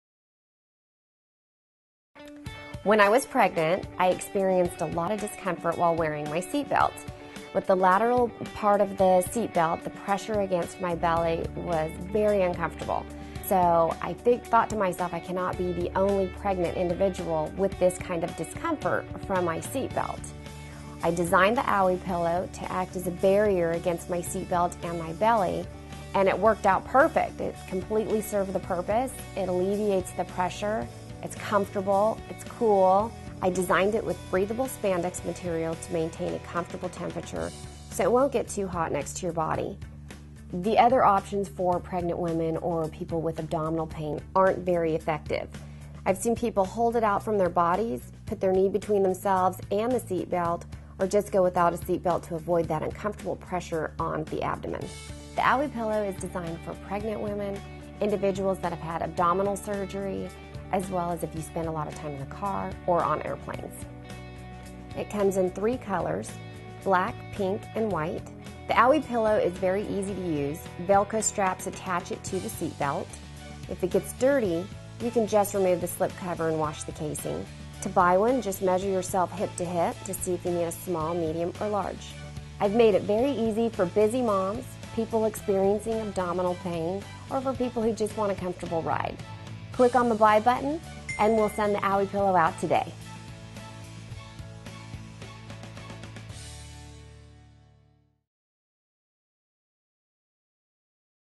Owie Pillow, seatbelt pillow, maternity pillow, lap pillow, pregnancy discomfort, abdominal pillow
Who is this product well-suited for?
Pregnant women, people who've had abdominal surgeries or wounds, or people who spend a lot of time in the car or on planes
"Protect the wound, surgical site or unborn child."
"Makes traveling much more comfortable and enjoyable.

"The other options for pregnant women or people with abdominal pain aren't very effective. I've seen people (hold it out from their bodies, put their knee between themselves and the seatbelt, or just go without a seatbelt to avoid that uncomfortable pressure on abdomen).

Very easy to use:
• Velcro straps attach it to seatbelt
• If it gets dirty, you can just remove the slip cover and wash the casing.
• I designed it with breathable spandex material to maintain a comfortable temperature (so it won't get too hot next to your body)
• It comes in three colors, Black, Pink and White.
• To buy one, just measure yourself hip to hip to see if you need a Small, Medium or Large
"I've made everything really easy for busy moms and others who just want a more comfortable ride. You can just click on the Buy button on this website and I'll send you your own Owie Pillow today.